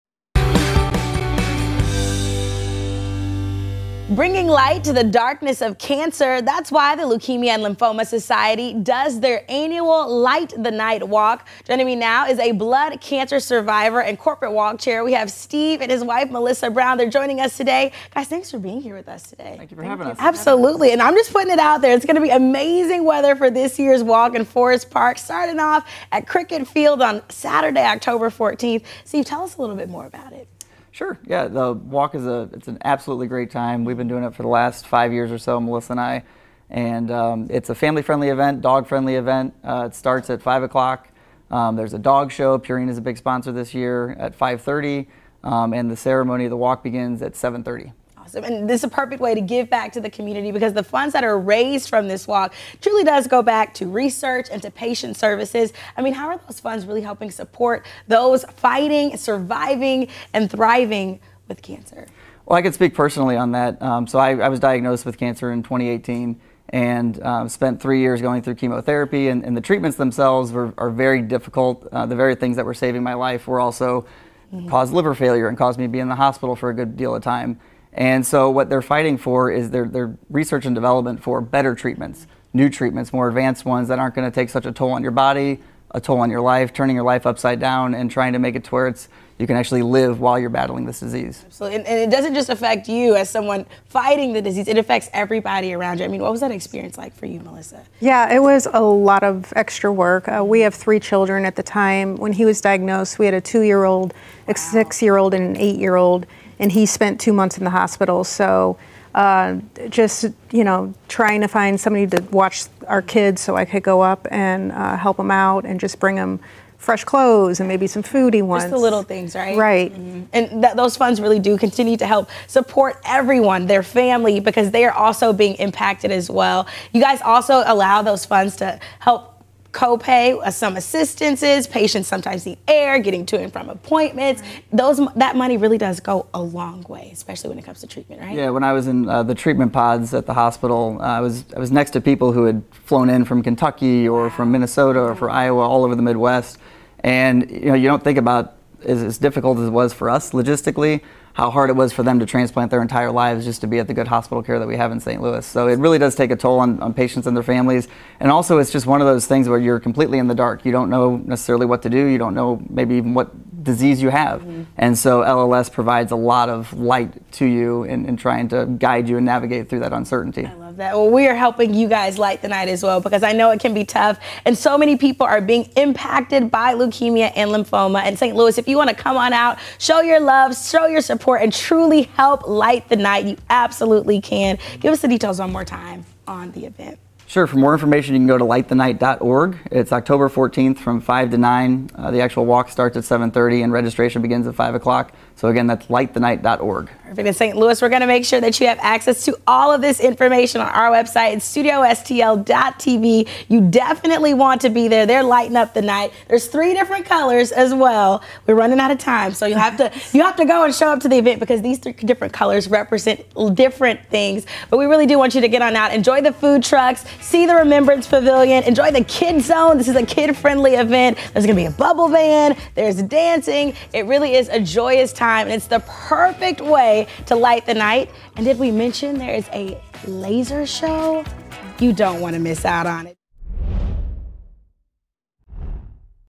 Bringing the light to the darkness of cancer
Every year, thousands gather for the Leukemia and Lymphoma Society’s Light the Night Walk.